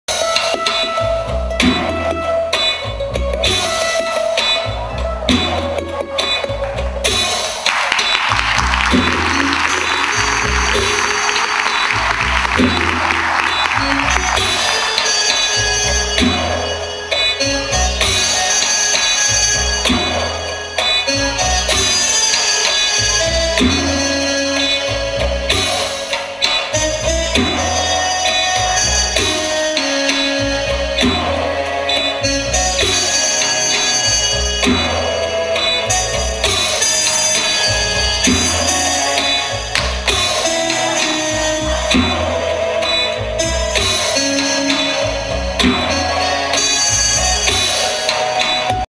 Saggio 2008 - Mimo
Saggio Spunkteatro 2008.Mimo Clown. Insegnante Alessandro La Rocca